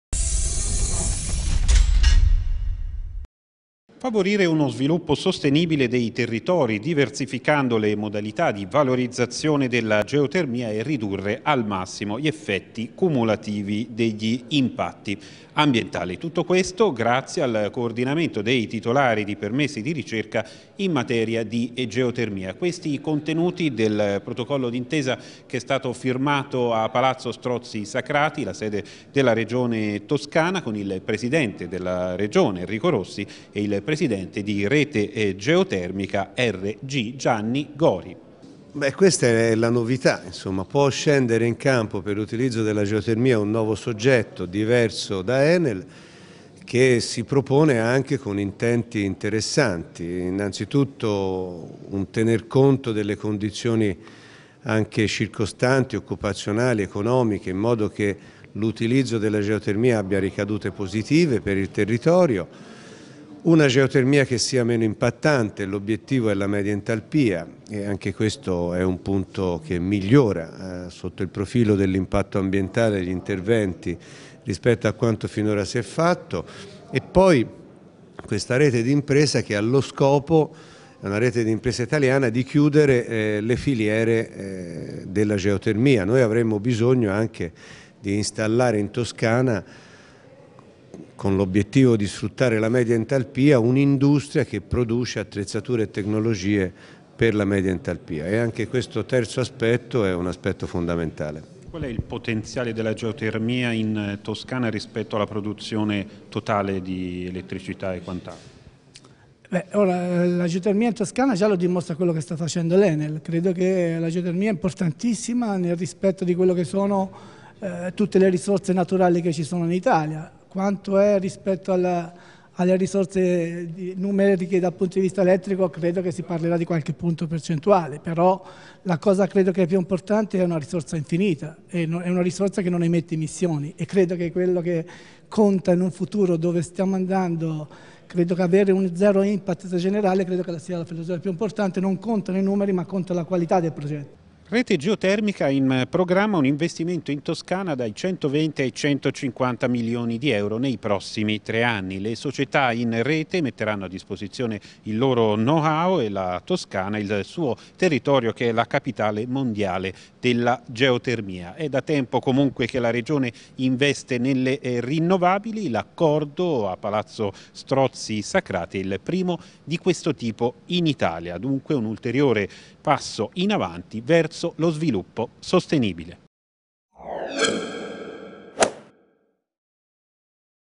Geotermia, intesa per produrre energia a impatto zero
E' il primo accordo del genere in Italia firmato da Regione e Rete Geotermica